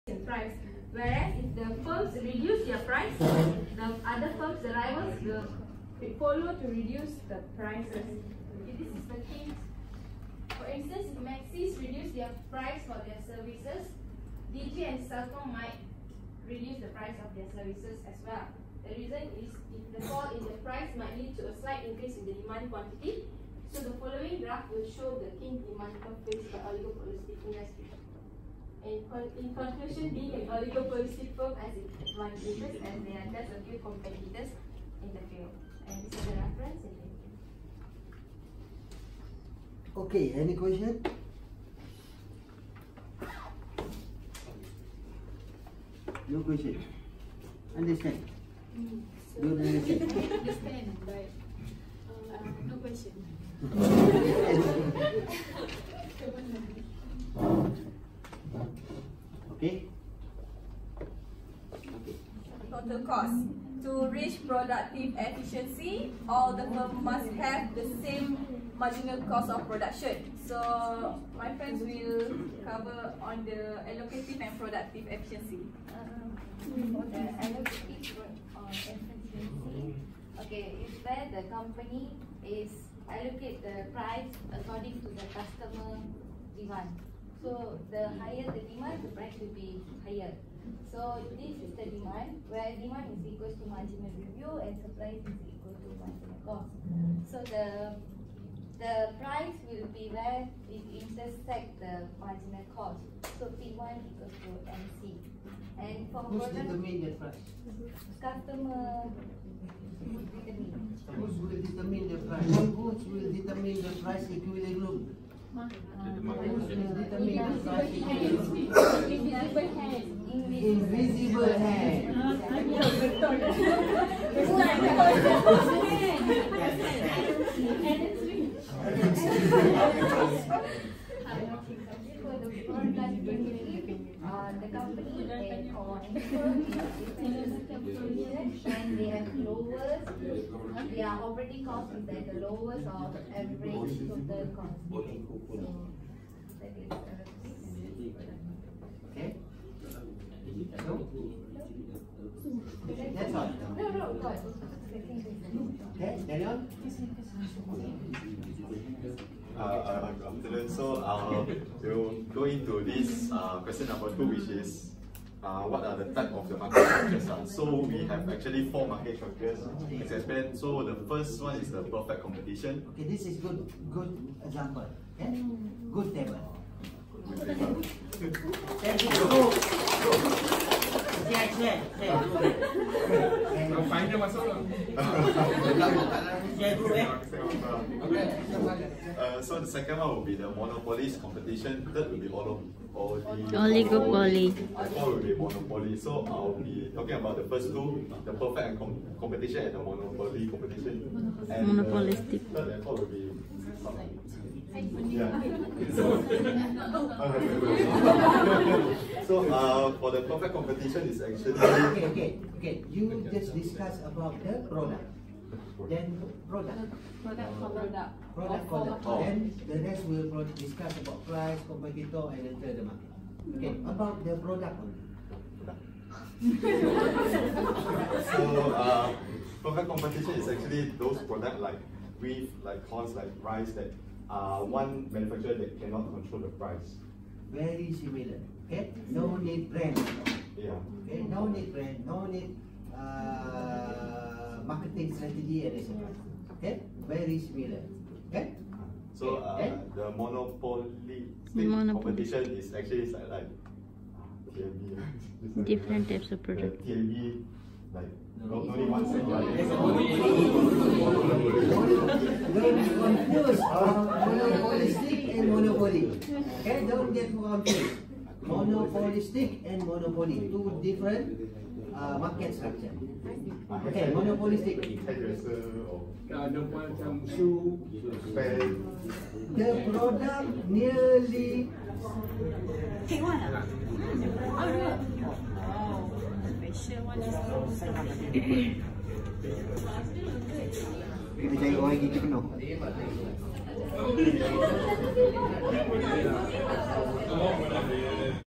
Economics assignment presentation of my MBA - semester 1
The group assignment is being presented by all of the groups at University Technology Malaysia.